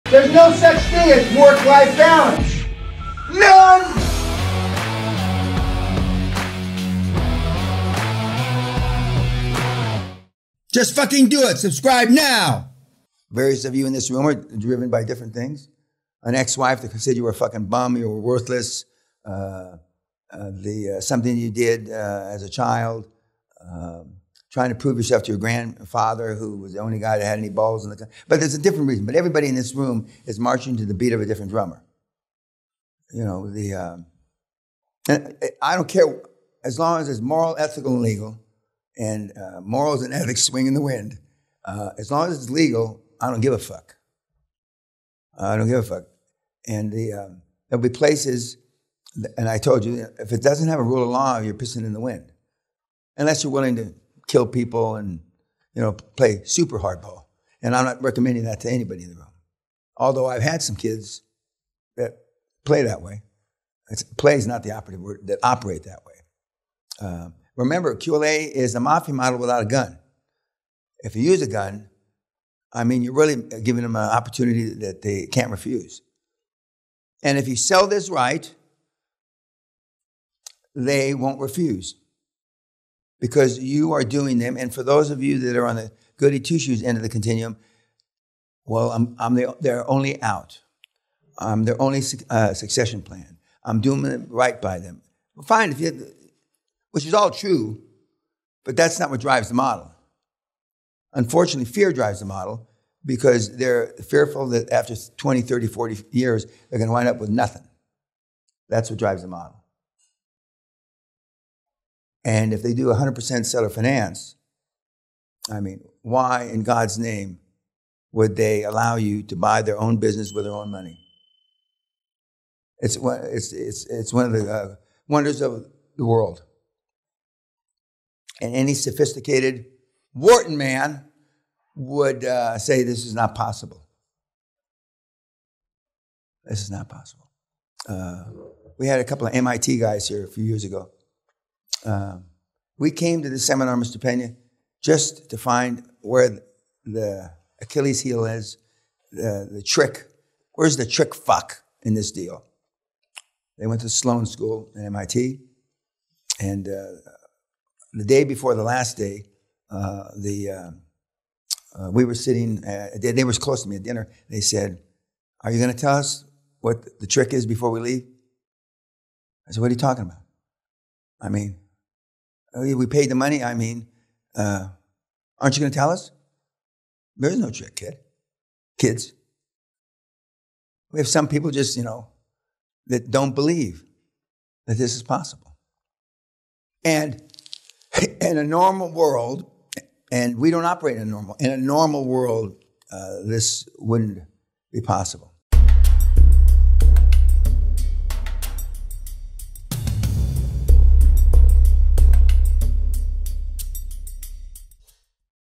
QLA IS A MAFIA MODEL WITHOUT A GUN | DAN RESPONDS TO BULLSHIT
The Trillion Dollar Man Responds To Bullshit - This is a highlight from the QLA Castle Seminar January 2022